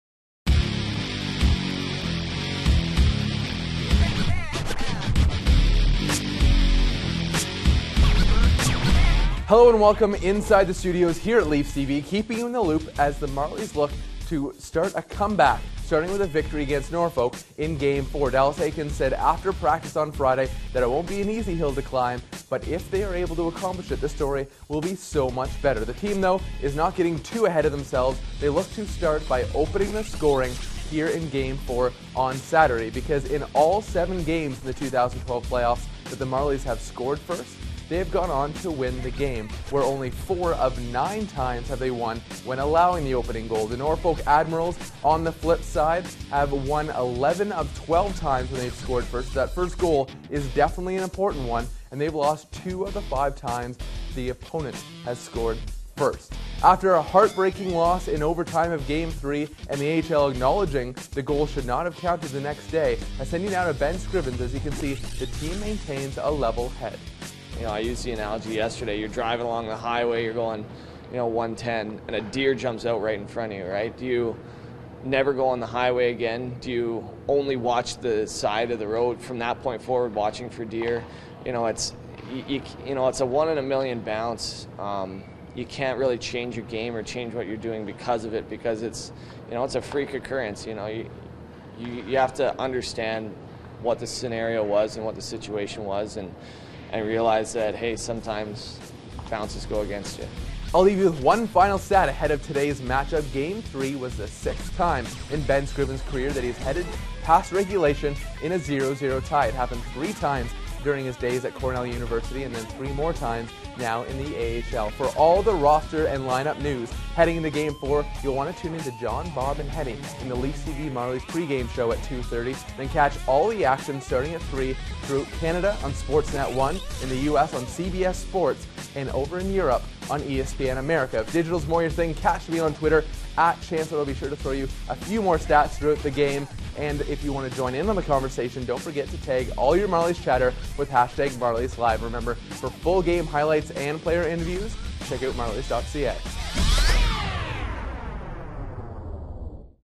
Marlies Live Pregame: Calder Cup Finals Game Four
Chansler keeps you in the loop with a look at scoring first, scoring second and a levelheaded Toronto attitude.